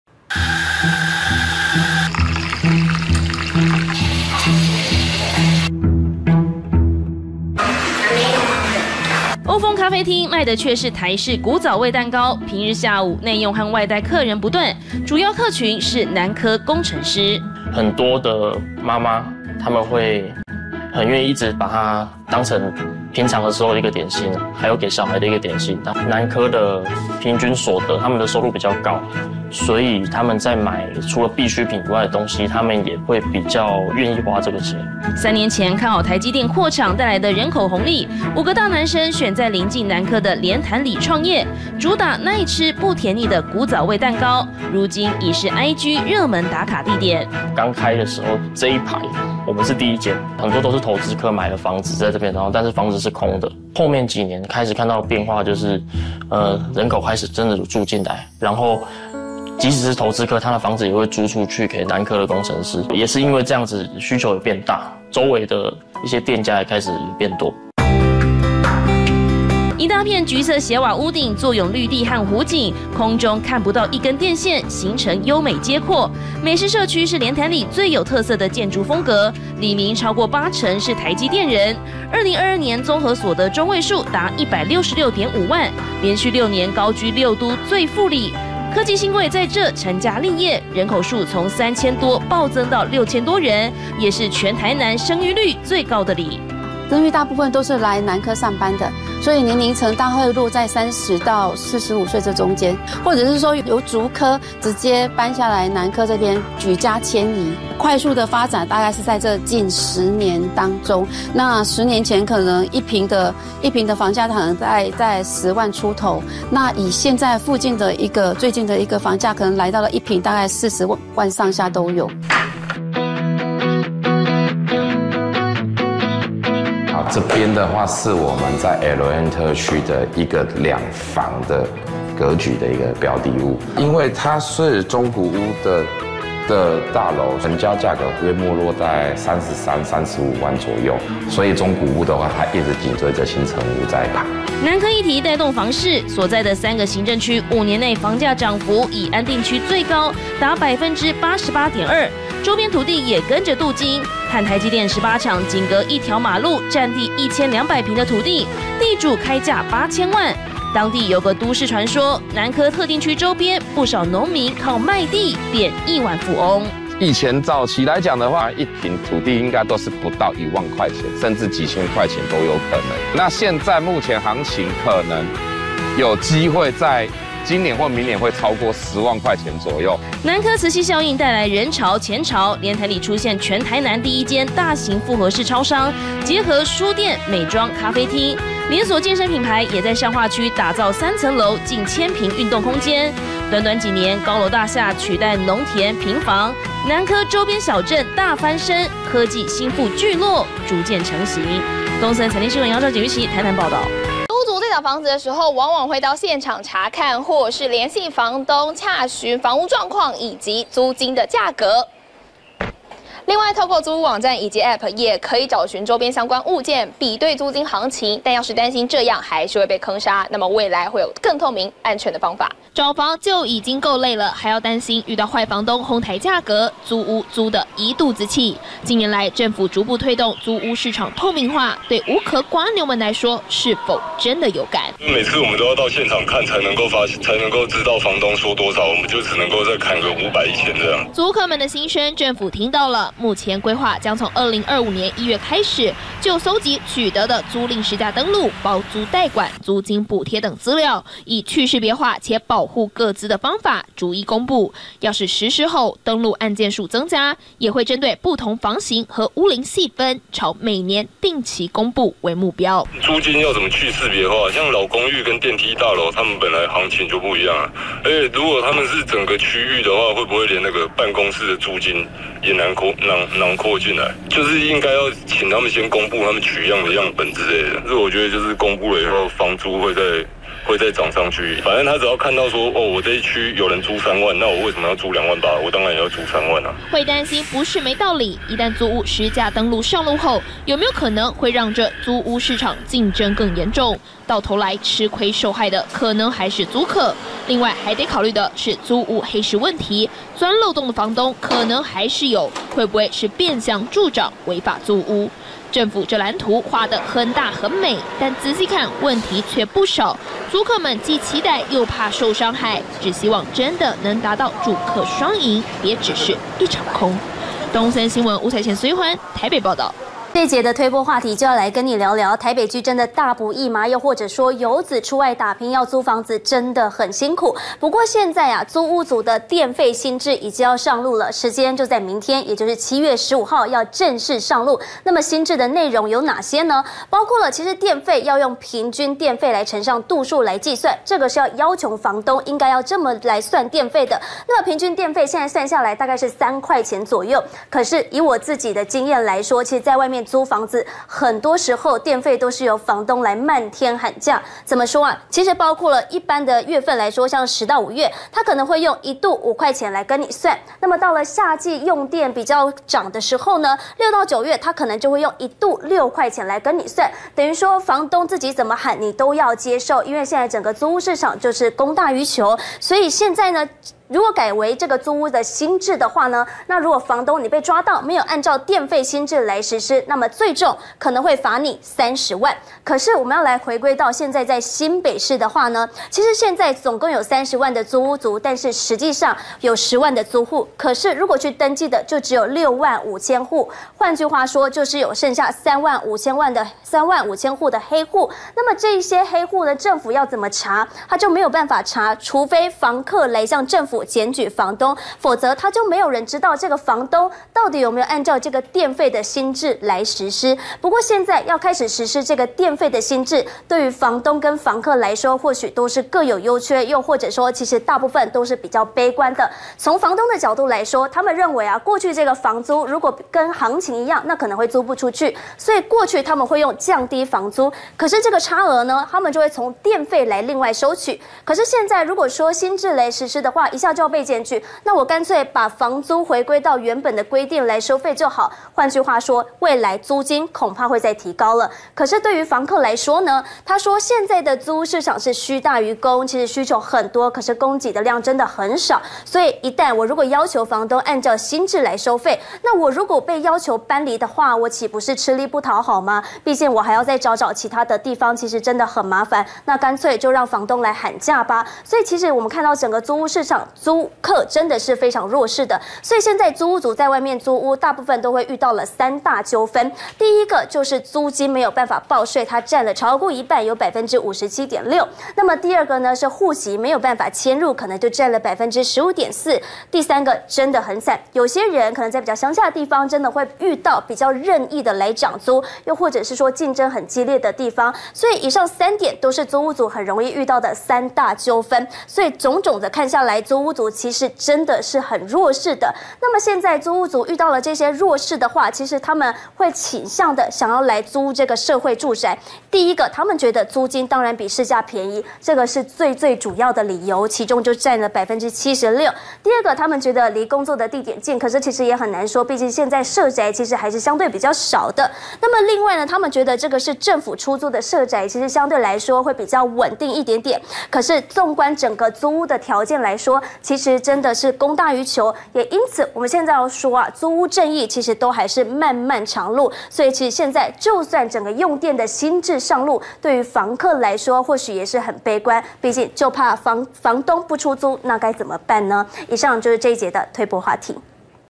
【地產王日報】8成里民是台積電員工 蓮潭里蟬聯六都最富里/明年1月將公布各區租屋行情 租客疑：真能全透明？/租屋電費新制7/15上路 須以「平均電價x度數」算電費
8成里民是台積電員工 蓮潭里蟬聯六都最富里
明年1月將公布各區租屋行情 租客疑：真能全透明？
【芷婕推播】租屋電費新制7/15上路 須以「平均電價x度數」算電費